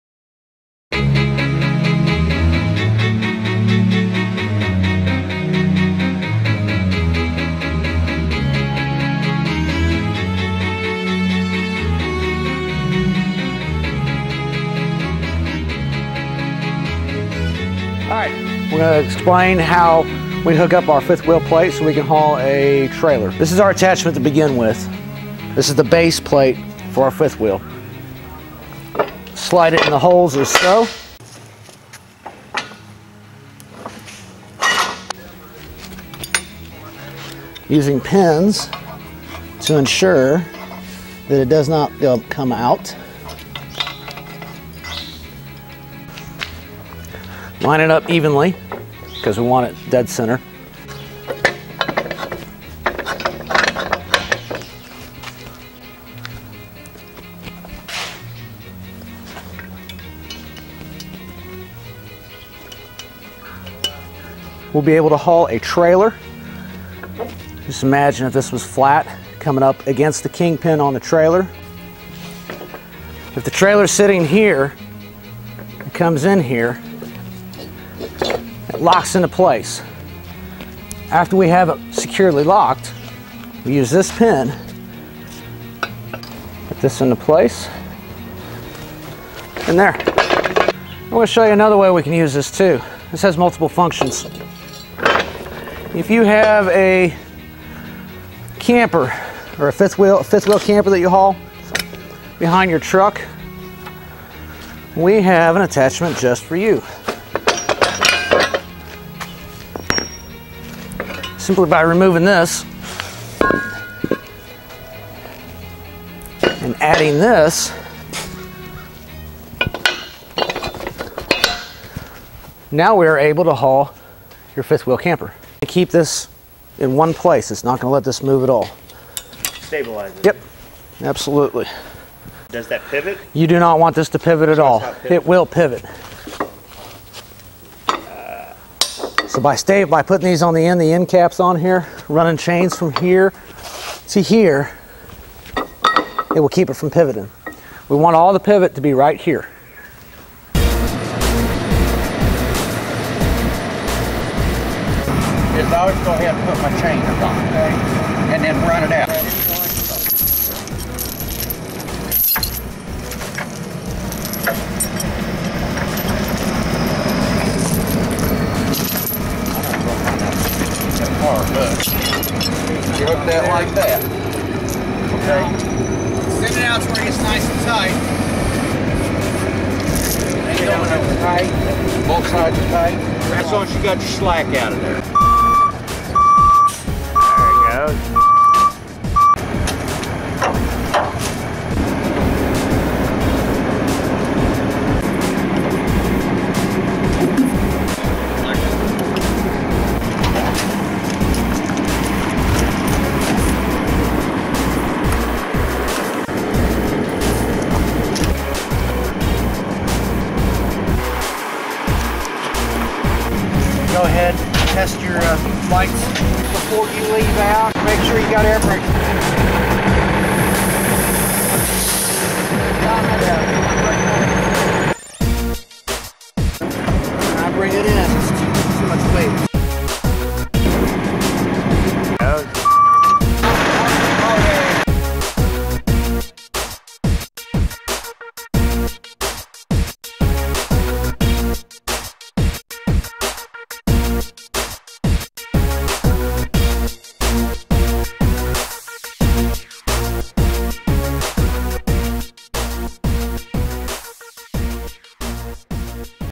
Hauling A 53 Ft. Trailer With A Single Axel Wrecker
Towing, Tow Trucks, and Tow Truck Drivers, we're having some fun now!! Rick, Leonard, Jason, and Don show how to use attachments to modify the wheel lift on a single axel wrecker so it can tow a 53 foot trailer.